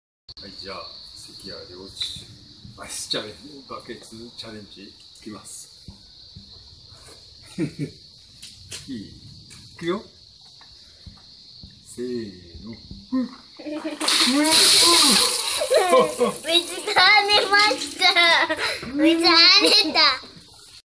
日本超馬名將「關家良一」回應「宇豪」指名, 執行冰桶挑戰。
世界超級馬拉松冠軍,在台灣東吳超級馬拉松24小時賽得過7次冠軍,一戰成名,來自日本的「關家良一」 回應 藝人跑者-南拳媽媽「宇豪」的點名,執行冰桶挑戰做愛心。響應跑者圈！